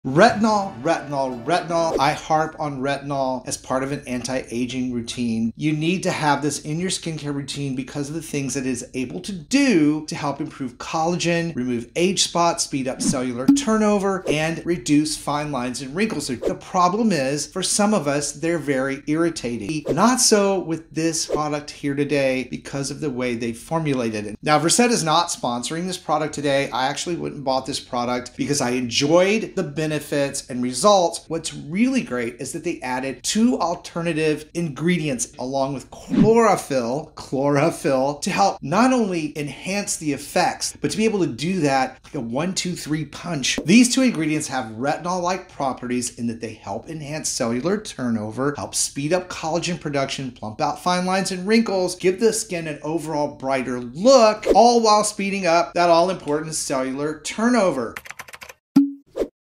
NEW RETINOL Serum Formula For Skin
A retinol product is an absolute must have in an anti-aging routine. Here is one that works for all skin types even sensitive skin.

Product I use featured in this video:
Versed Gentle Retinol